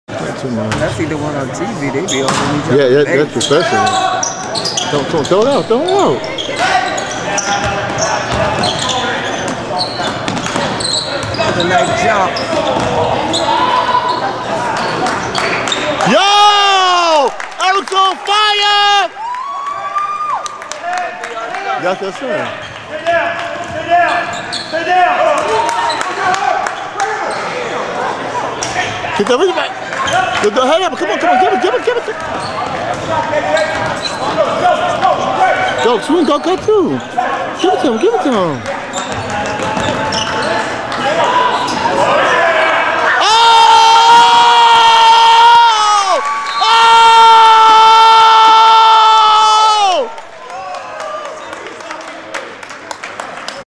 Eric is on Fire! Ends the 3rd quarter with a pair of threes. One of the best Class of 2018 shooters!
Eric in a JV game 2015 freshman year. Conwell Egan Catholic. Class of 2018 shooter.